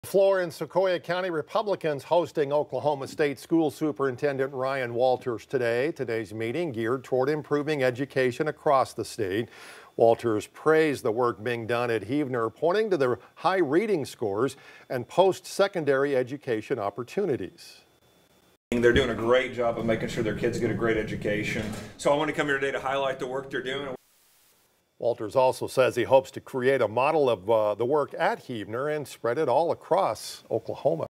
Oklahoma State Superintendant visits Heavener schools
Walters praised the work being done at Heavener, pointing to high reading scores.